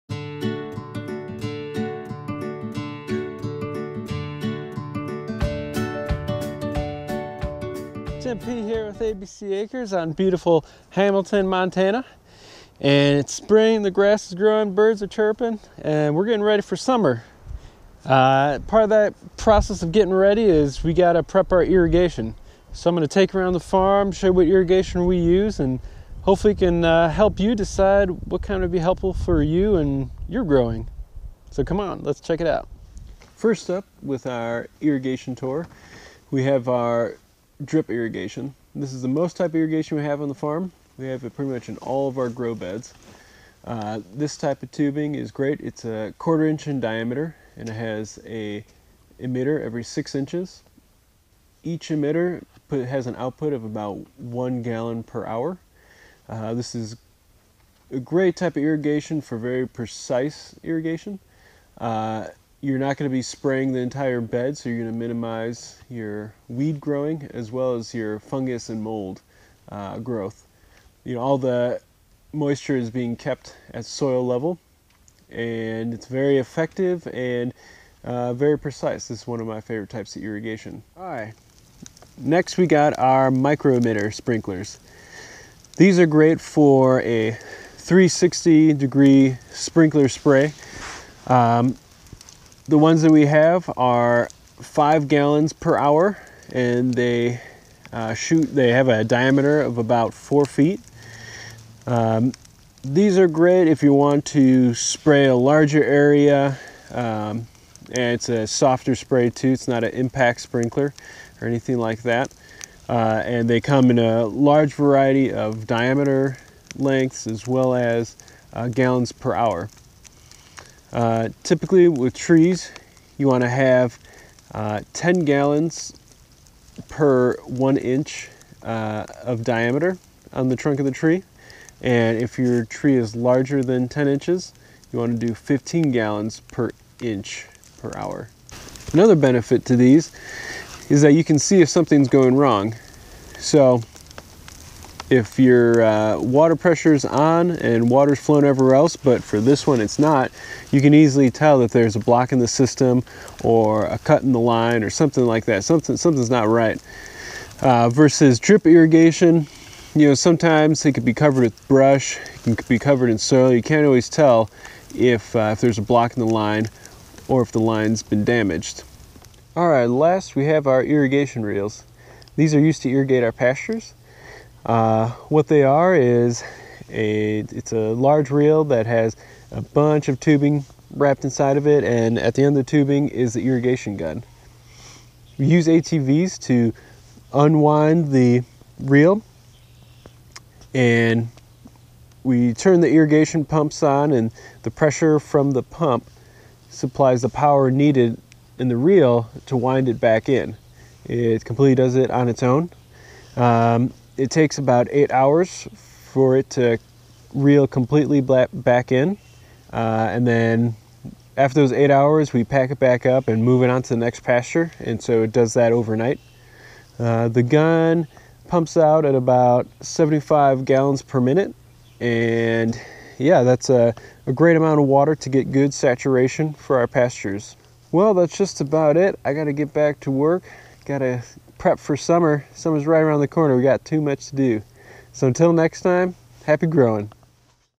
Farm Irrigation 101: ABC Acres - Episode #170
Join ABC acres, a Permaculture Farmstead in Hamilton, MT as we show you our most common forms of irrigation on the farm